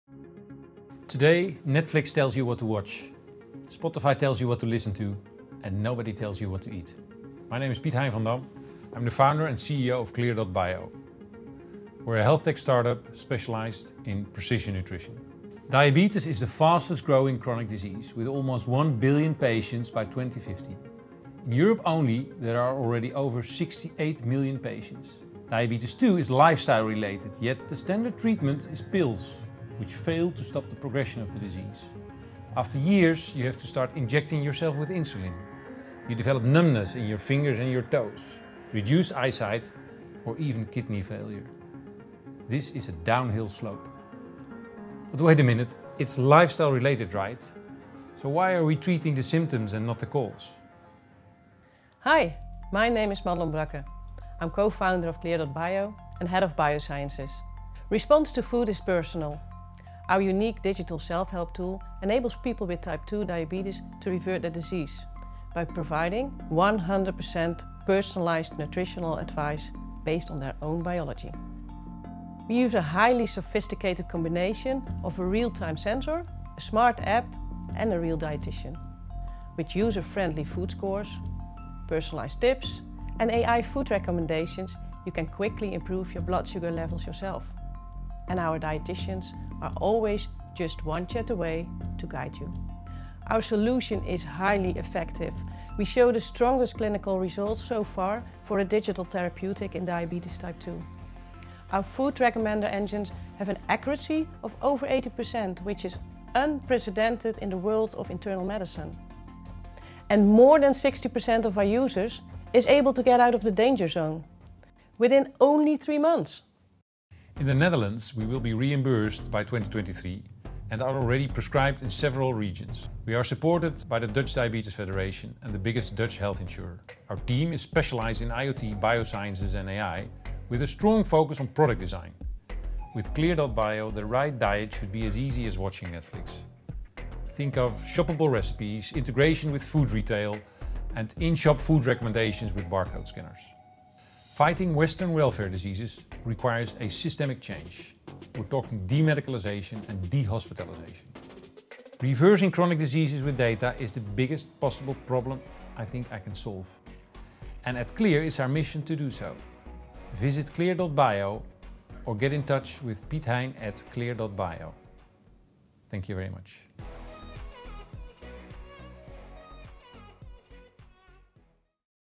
The elevator pitch of Clear.bio
Curious about who we are, what we do and what drives us? Watch this video and get to know our story.

Today, Netflix tells you what to watch, Spotify tells you what to listen to, and nobody tells you what to eat.

My name is Piet Hein van Dam. I am the founder and CEO of Clear.bio. We’re a healthtech startup specialized in precision nutrition.

Diabetes is the fastest growing chronic disease, with almost 1 billion patients by 2050. In Europe only, there are now over 68 million patients.

DT2 is lifestyle related, yet the standard treatment is pills, which fail to stop progression of the disease. After years, you have to start injecting yourself with insulin. You develop numbness in your fingers and your toes, reduced eyesight or even kidney failure.

It’s a downhill slope.

But wait, it’s lifestyle related, right? So, why are we treating the symptoms, and not the cause?

Hi, my name is Madelon Bracke, I am co-founder of Clear.bio and head of biosciences.

Response to food is very personal. Our unique digital self help tool enables people with type 2 diabetes to revert their disease by providing 100% personalized nutritional advice based on their own biology.

It’s a very sophisticated combination of a real-time sensor, a smart app and a real dietician.

With user-friendly food scores, personal tips and AI food recommendations, you can quickly improve your blood sugar levels yourself. And our dieticians are always just one-chat-away to guide you,

Our solution is highly effective. We show the strongest clinical results so far for a digital therapeutic in diabetes type 2.

Our food recommender engines have an accuracy of over 80%, which is unprecedented in the world of internal medicine.

And more than 60% of our users is able to get out of the danger zone within 3 months.

In the Netherlands, we will be reimbursed  in 2023 and are already prescribed in several regions. We are supported by the Dutch Diabetes Association and the biggest Dutch health insurer.

Our team is specialized in IoT, bio sciences and AI, with a strong focus on product design. With Clear.bio, the right diet should be as easy as watching Netflix. Think of shoppable recipes, integration with food retail and in-shop food recommendations with barcode scanners.

Fighting western welfare diseases requires a systemic change. We’re talking de-medicalisation and de-hospitalization.

Reversing chronic diseases with data is the biggest possible problem we think we can solve. And at Clear it is our mission to do so.